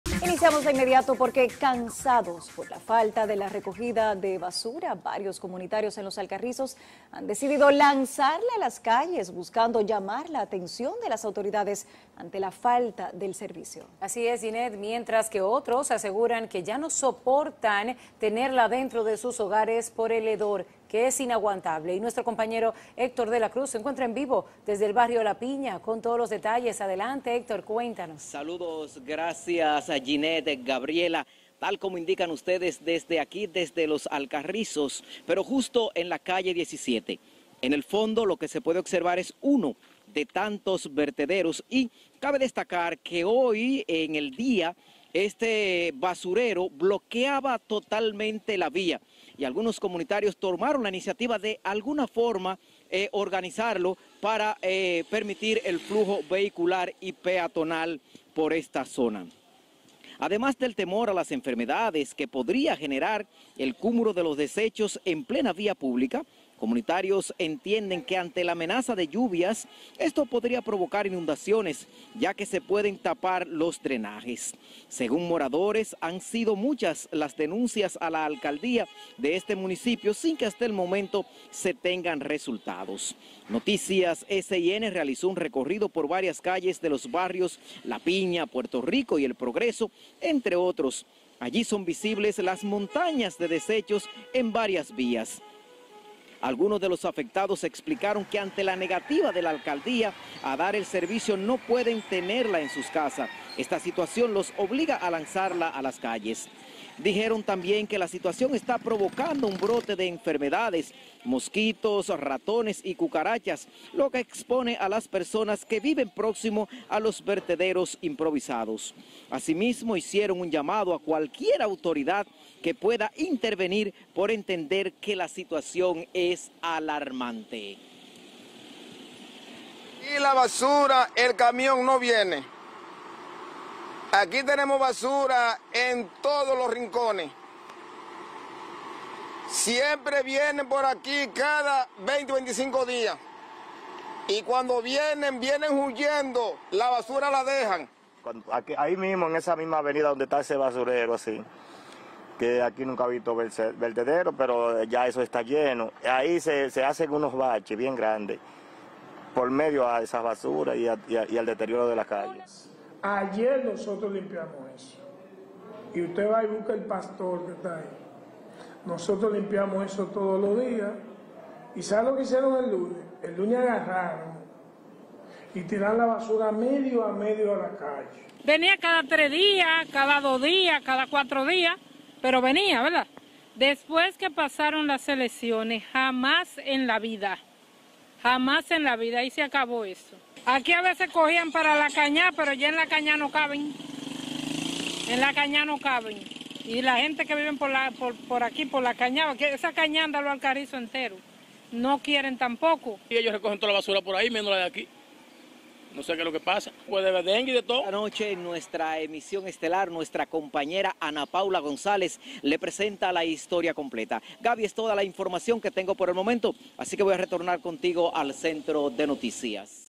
Arropados por la basura residentes en Los Alcarrizos protestan por falta de recogida
El temor a las enfermedades que podría generar el cúmulo de los desechos en plena vía pública, permanece en los comunitarios de Los Alcarrizos, quienes entienden que ante la amenaza de lluvias, se podría provocar inundaciones que pueden tapar los drenajes.

Según moradores han sido muchas las denuncias a la alcaldía de este municipio sin que hasta el momento se tengan resultados.